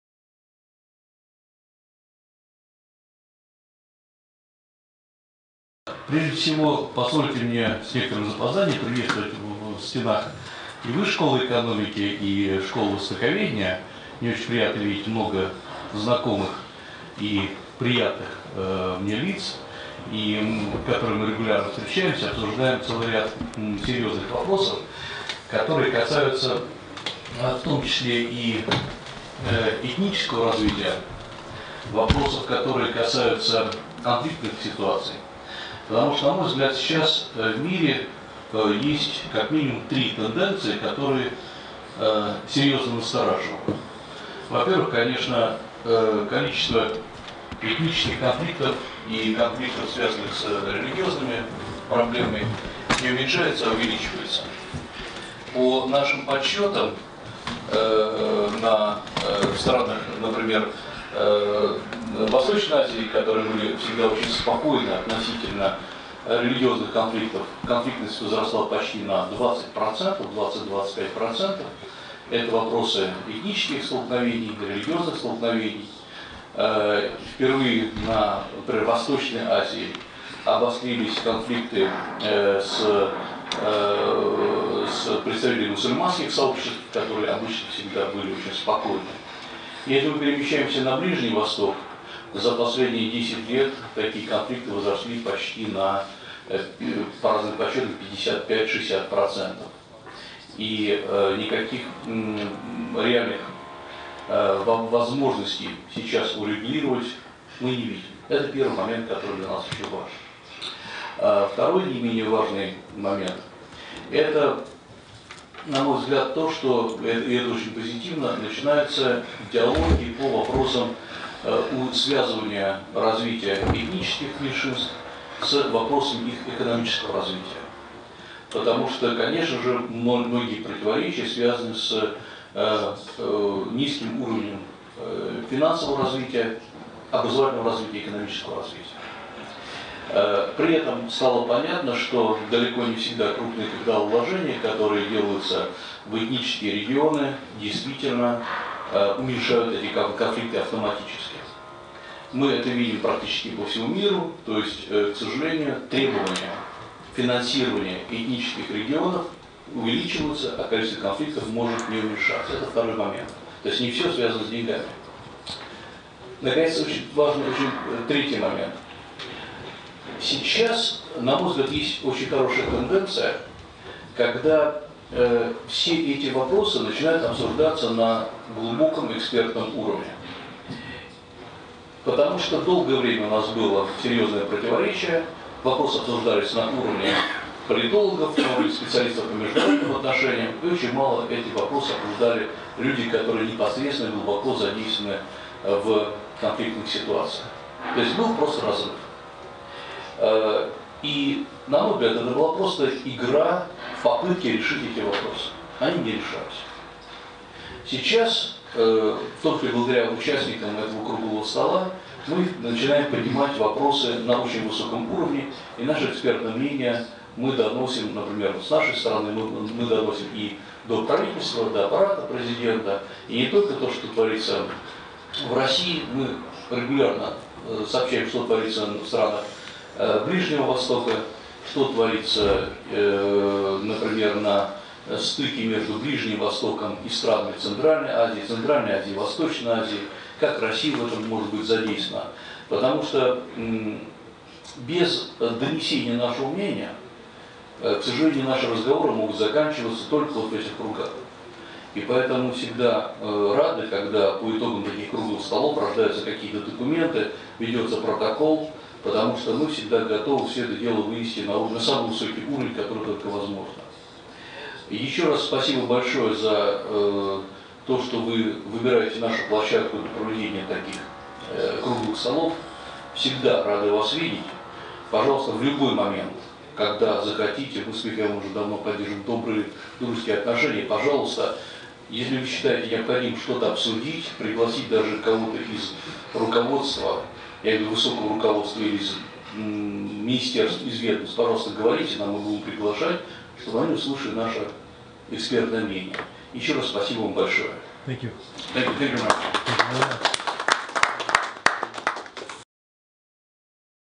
Алексей Маслов, директор Школы Востоковедения НИУ-ВШЭ. Москва,, 31.05.2018
Выступление, приветственное слово. Конференция "Положение этнических и религиозных меньшинств в условиях многонационального региона". 2 часть конференции, Москва, Школа востоковедения НИУ-ВШЭ, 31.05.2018